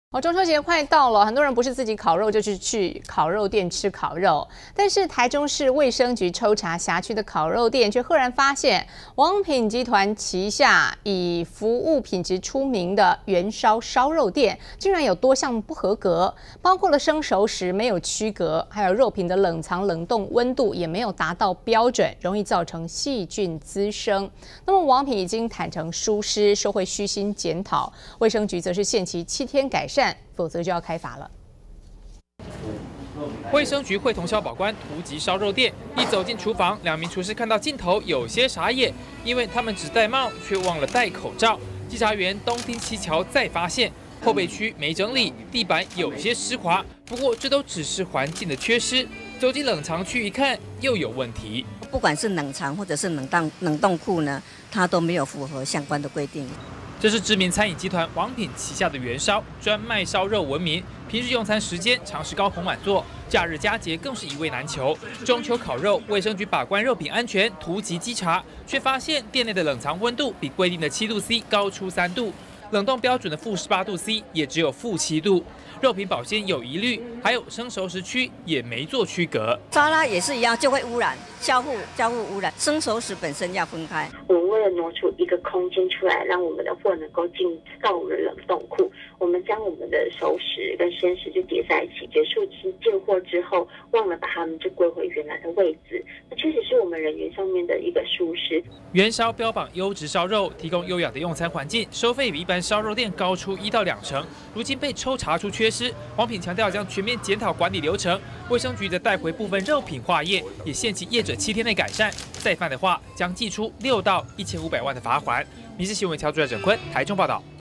原燒 衛生大曝光 娛樂新聞 臺中「原燒」稽查不合格 王品原燒坦承疏失
娛樂新聞 臺中「原燒」稽查不合格 王品原燒坦承疏失